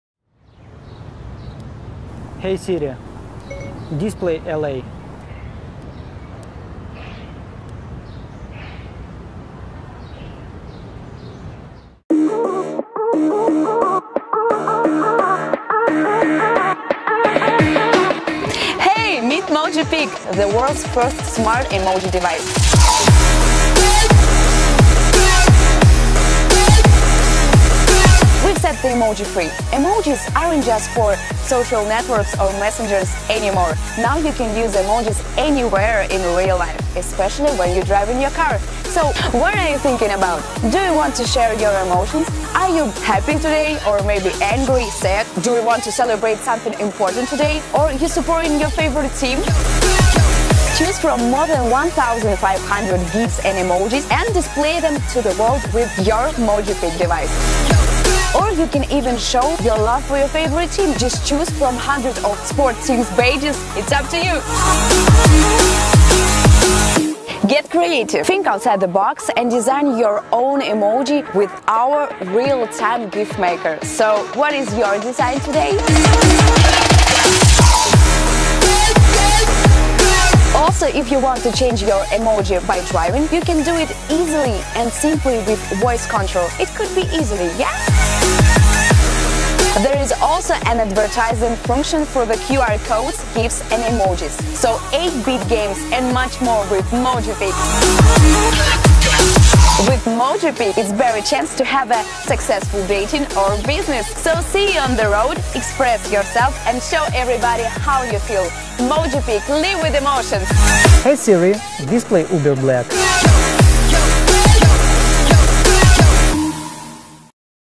Mojipic: First Voice-Controlled Emoji Car Display
Tell a tailgater to back off, represent your sports team or just have fun with GIFs and EMOJIs.

Overview
- Voice Control: Integration with Apple and Google, enables you to display images without being distracted on the road
- More than 1500 GIFs, Smiles & Emojis. Baseball Teams, NFL, NBA, NHL, Soccer, flags of countries, national symbols and more.
- QR Code: Want to share your story or business while on the road?
- Real-Time Drawing & GIF Maker: Create your own drawings, images or animated GIFS
- 8-Bit Games: Keep passengers entertained with pre-installed 8-bit games.

Meet
Introducing Mojipic. The world’s first smart voice emoji device.

Use Siri or Google Assistant to show everyone on the road how you feel with fun emojis and GIFs. Now, you can make driving more fun than ever!

Send smileys out to considerate drivers, show your love for your favorite sports team or send love hearts to that cutie in the car behind.

Express yourself and get creative on the go!

Day or night, low-light or super-bright light. Everyone will be able to see Mojipic. It’s bright enough to be seen, without being blinding.

Sometimes you just don't have time to get onto your phone, not to mention how stupid it is while driving. You can use Siri or Google to change EMOJIs or GIFs while you drive.